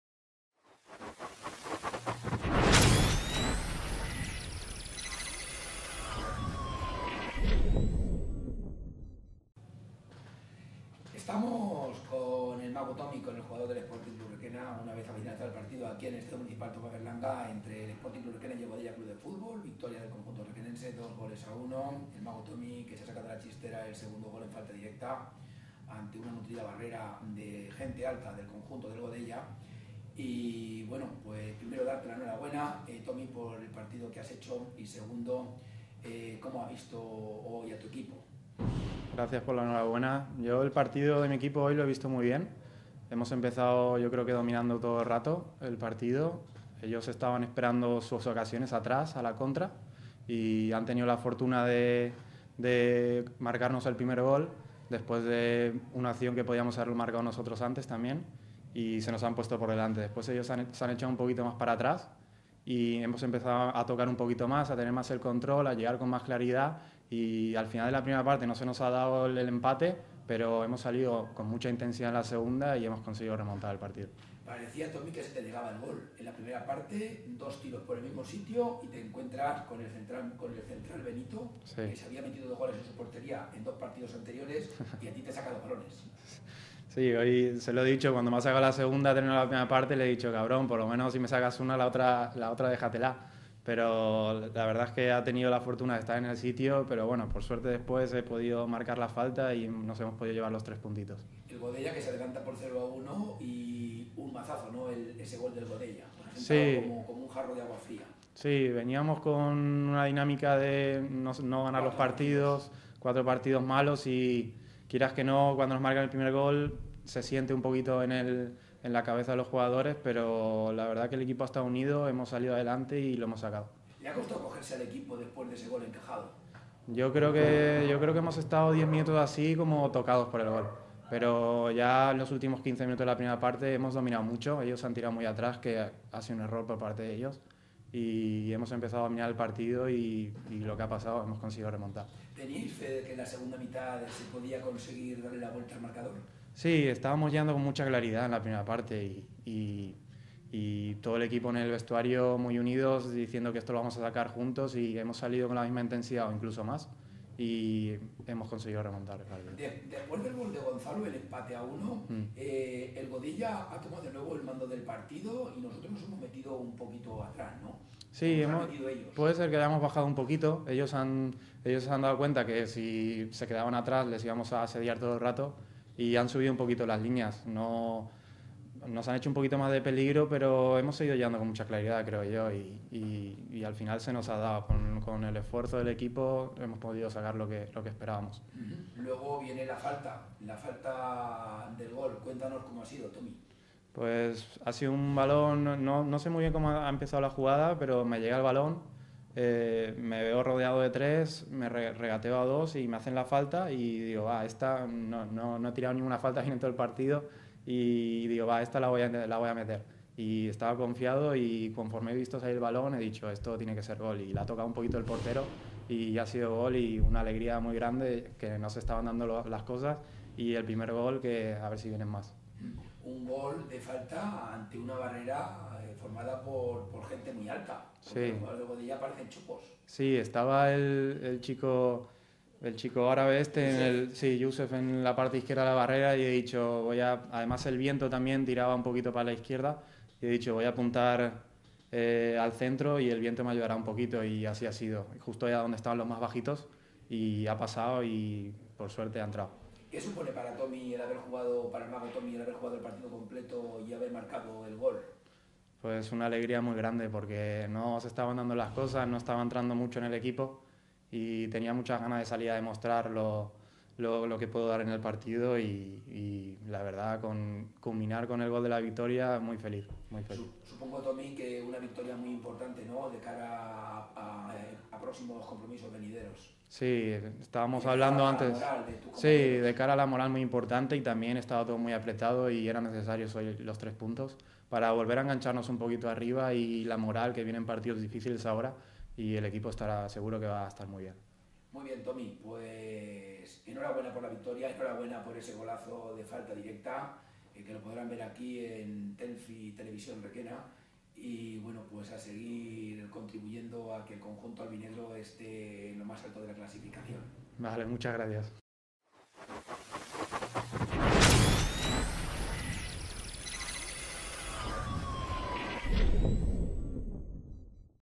Entrevista Post partido contra Godella jugador Tomás Marcelo Ratto Tomy
Entrevista al jugador del Sc Requena,  Tomás Marcelo Ratto   Tomy , , tras la victoria en casa frente al CD Godella por 2 goles a 1